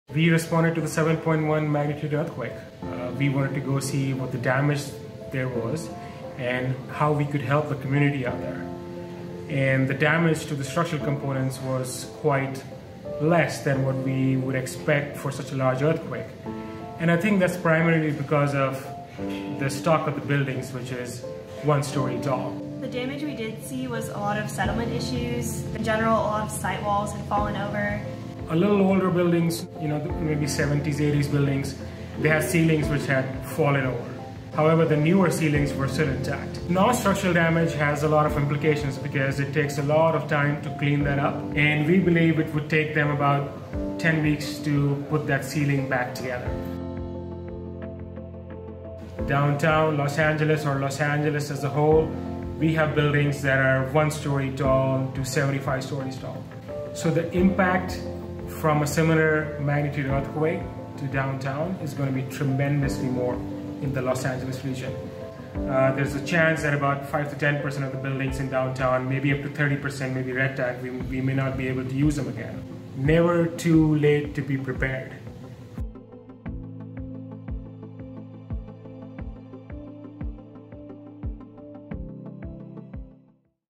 Magnitude 7.1 Ridgecrest Earthquake
RIDGECREST – Structural engineers from Miyamoto International conducted earthquake reconnaissance in the Searles Valley immediately after California’s magnitude 6.4 and 7.1 earthquakes on July 4 and 5. From an engineering perspective, Ridgecrest and Trona, the small towns most impacted by the earthquakes were lucky. Both have mostly one-story buildings that withstood the earthquakes reasonably well.  Large urban areas would not be so lucky. And even in Ridgecrest and Trona, owners are finding that both structural and non-structural damage can take a long time to clean up and repair, putting closed businesses in financial stress.

A relatively new city in the high desert of California’s Kern County, Ridgecrest has mostly one-story, light-framed buildings with a smattering of one-story reinforced concrete block (CMU) buildings with wood framed roofs.  Buildings made of wood or light steel are lighter than concrete or structural steel buildings, so they experience less force during an earthquake – and less damage.  Most of the residential damage, if any, was to the non-structural components such as fence walls and canopies. We also assessed two major grocery stores and saw damage like ceiling panels dislodged or structural component issues, which were damaged while doing the job they were designed to perform.

Trona, a smaller city that lists mining as its major industry, had residential damage to older one-story houses with brick chimneys, which fell into houses. Chimneys are often the first to go. Fence walls that were damaged were built without reinforcing. Most of the damage was due to differential soil settlement caused by the earthquakes.

While Ridgecrest and Trona experienced minimal damage, this will not be the case in areas like Los Angeles or the Bay Area when an earthquake of this magnitude strikes there. This is because those urban centers have buildings of all types, sizes, heights, vintage and construction methods. “There’s a chance that 5 to 10 percent of the buildings in downtown Los Angeles, maybe up to 30 percent, will be red tagged,” said structural engineer Deepansh Kathuria. “We need to prepare now.”